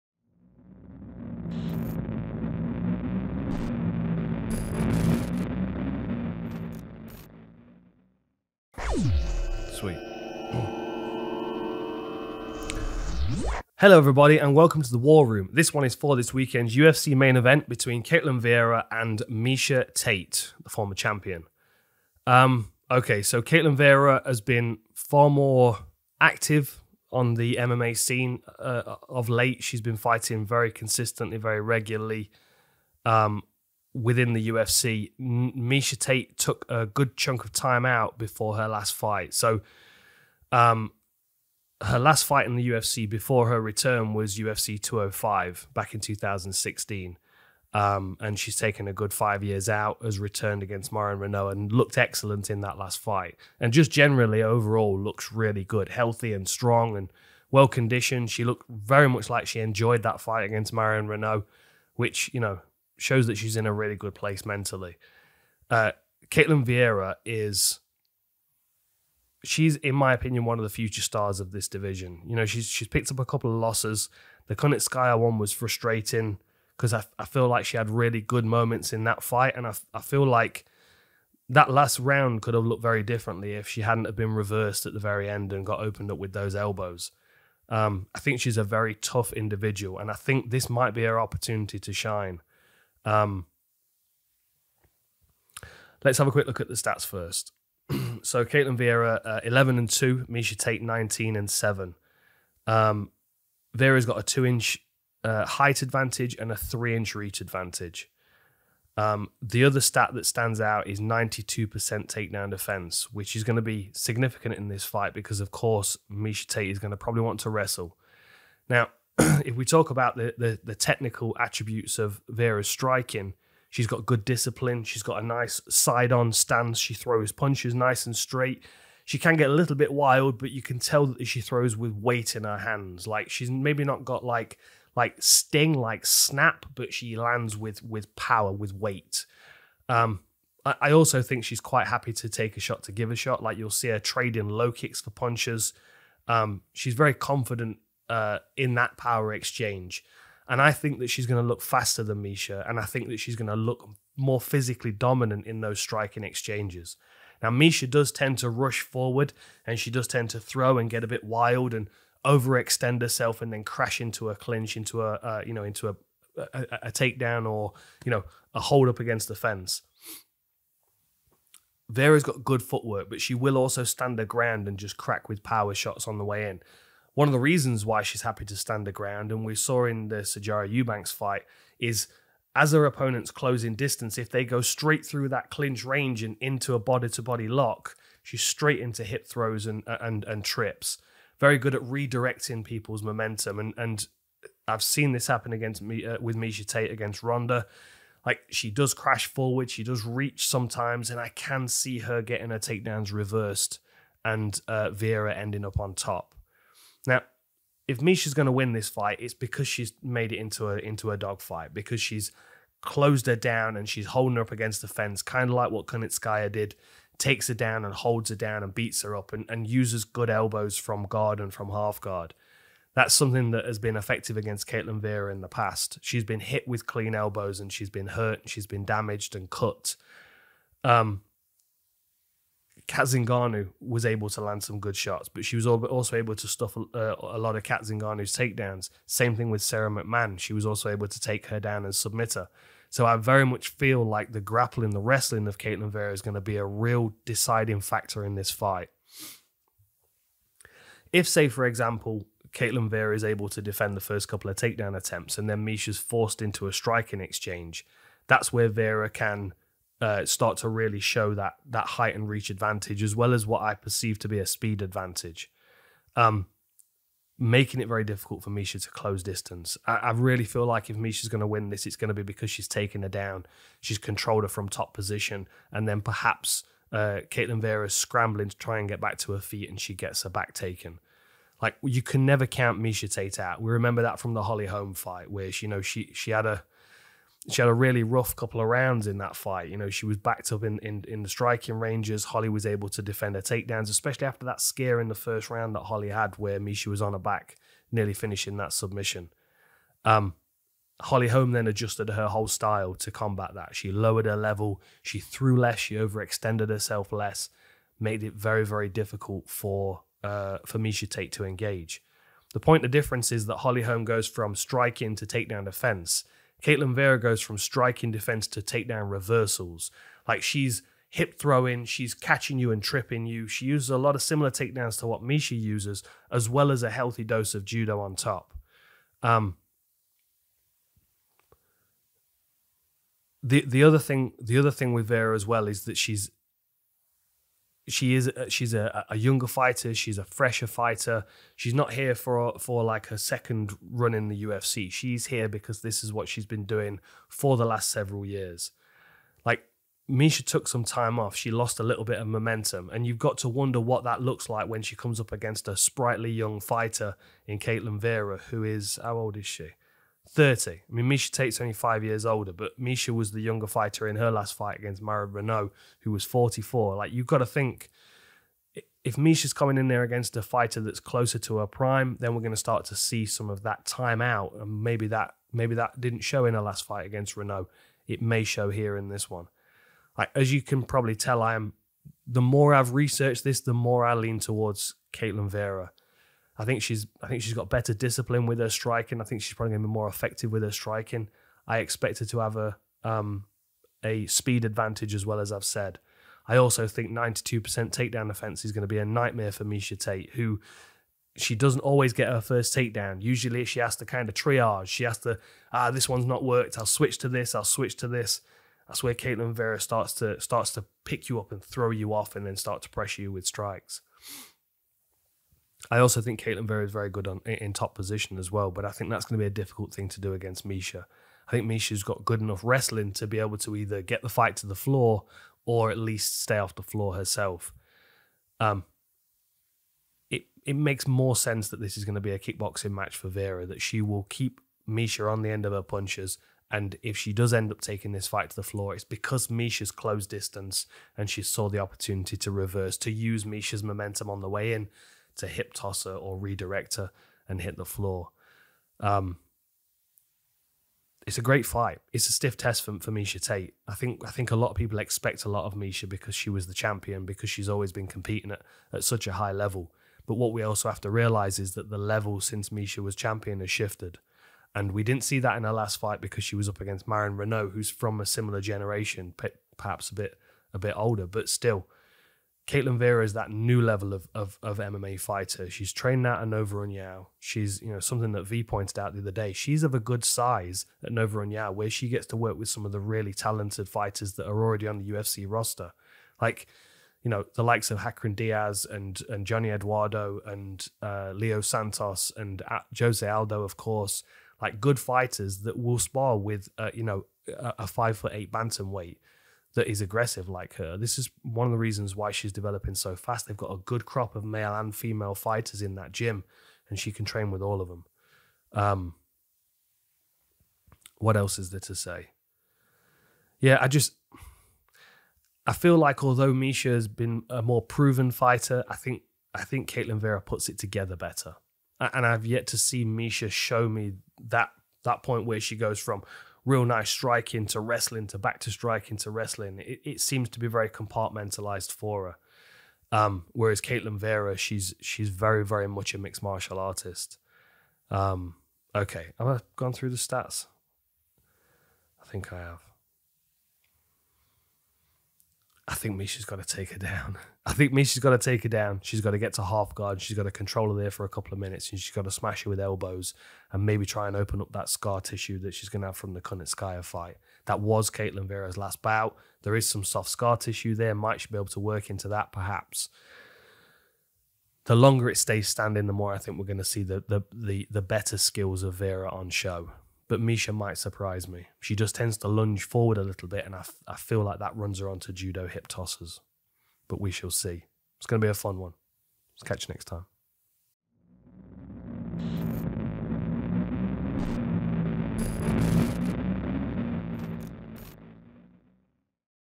Ketlen Vieira vs Miesha Tate | The War Room - Dan Hardy Breakdown EP. 153
The War Room, where Dan Hardy breaks down the up coming UFC Main Event between the former Bantamweight Champion Miesha Tate and the future star Brazilian Fenomeno, Ketlen Vieira.

For Outlaws, Full Reptile offers a wide range of Training and Lifestyle clothing that provides quality and comfort for ninja acrobatics or everyday wear. Check out the shop!